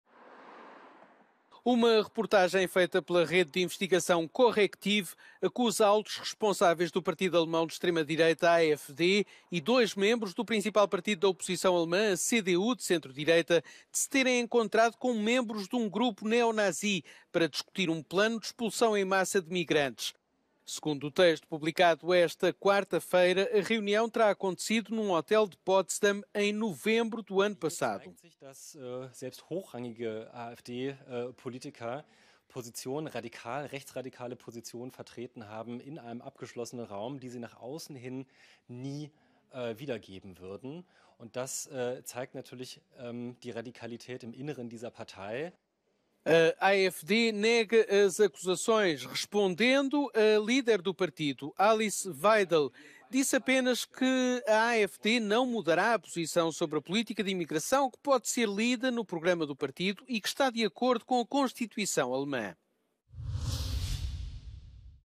Membros da AfD e da CDU terão encontrado neonazis em reunião secreta
Reportagem explosiva de uma rede de investigação revela supostas ligações entre membros da direita e extrema-direita alemã e grupúsculos extremistas.